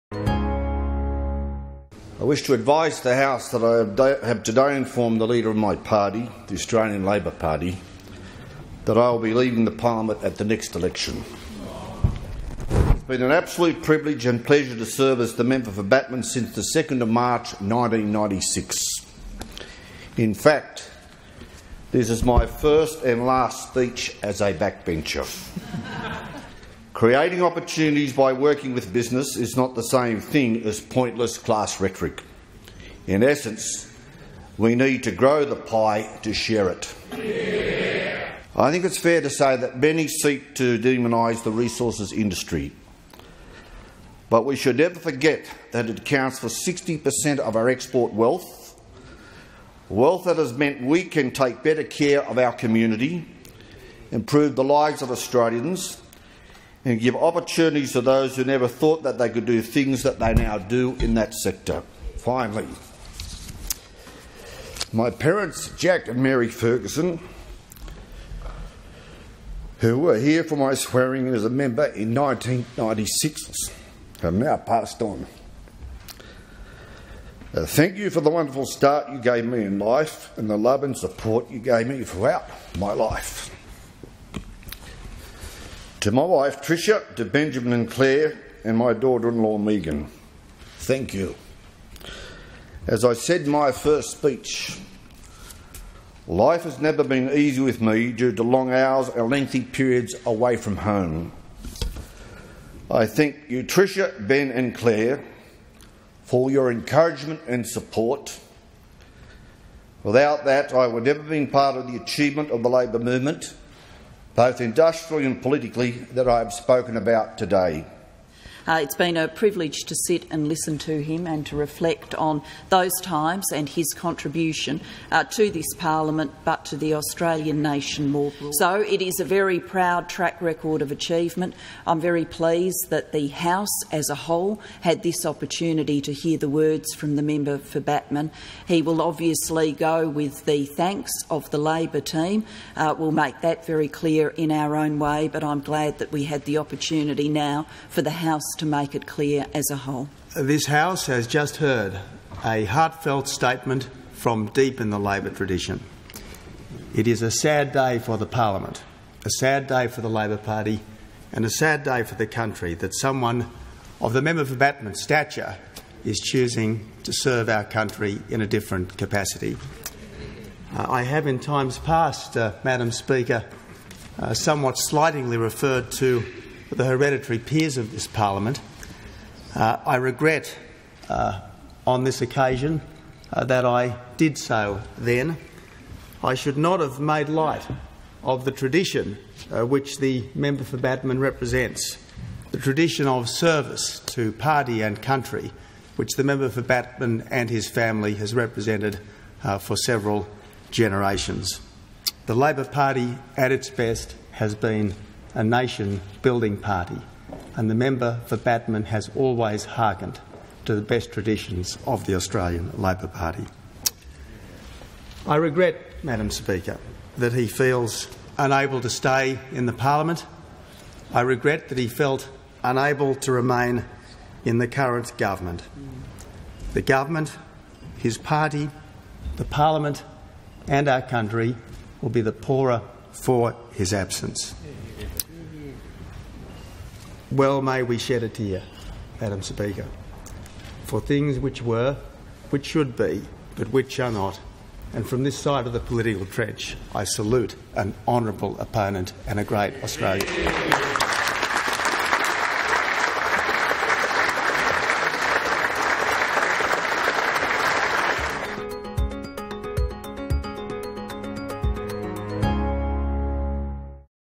Ferguson announces retirement from politics
Labor stalwart Martin Ferguson has told parliament that he will retire at the September 14 election.
Mr Ferguson, who has held the seat of Batman in Victoria since 1996, addressed the House in his "first and last speech as a backbencher".